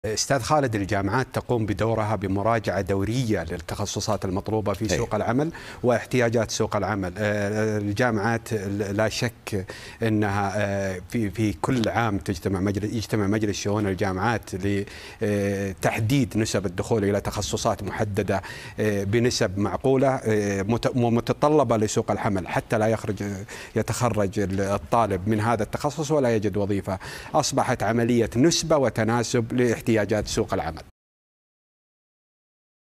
مستشار الموارد البشرية: الجامعات تراجع بشكل دوري احتياجات سوق العمل
نشرة الرابعة | مستشار الموارد البشرية: الجامعات تراجع بشكل دوري احتياجات سوق العمل وعليه تحدد نسب الدخول لتخصصات معينة في السعودية